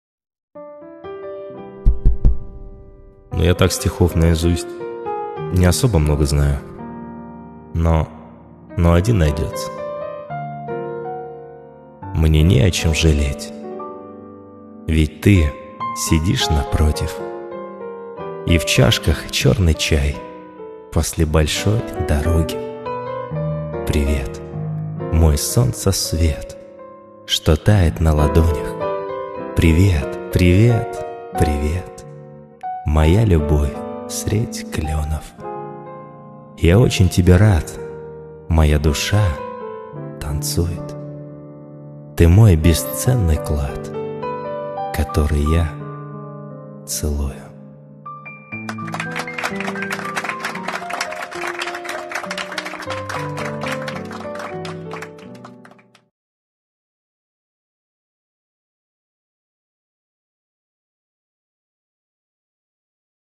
Привет | Читает автор: Станислав Аспин
Привет

Мне не о чем жалеть,
Ведь ты сидишь напротив,
И в чашках черный чай,
После большой дороги,

Привет, мой солнца свет,
Что тает на ладонях,
Привет, привет,привет!
Моя любовь средь кленов,

Я очень тебе рад,
Моя душа танцует,
Ты мой бесценный клад,
Который я целую.